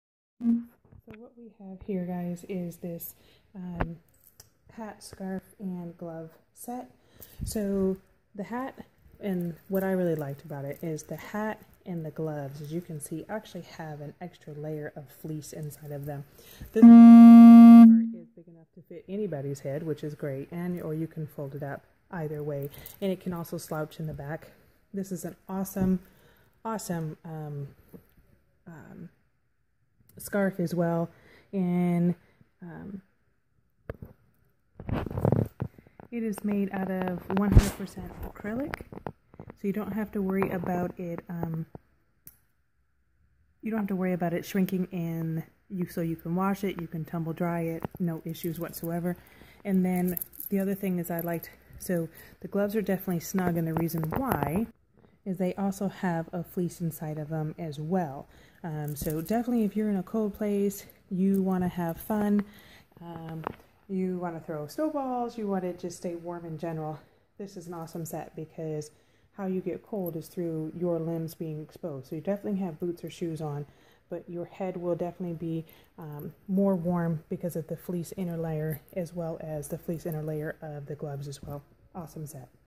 N'Ice Caps Women's Solid Cable Knit Hat/Scarf/Gloves Accessory Set
Super cute and WARM Hay, scarf, and glove set!  I am getting ready to head to Tahoe and I live in Southern Cali so you know I am not ready or used to the cold!  So, what better way to keep me warm that this great set!  it is made of acrylic and poly and it isn't itchy at all.

The hat is made with a slouch style - so you can stay in style yet be warm :) It also has a fleece lining to it which is awesome to help to keep the heat in.

The scarf is a nice length and able to truly wrap around your neck and tuck it into your coat if you choose.

The gloves have a nice fit to them and are also lined with fleece.  So, no worries, the knit is not the only thing keeping you warm there, the fleece is a huge help on the gloves!

The construction of this set is well made with good solid materials.

I received this product at a reduced price.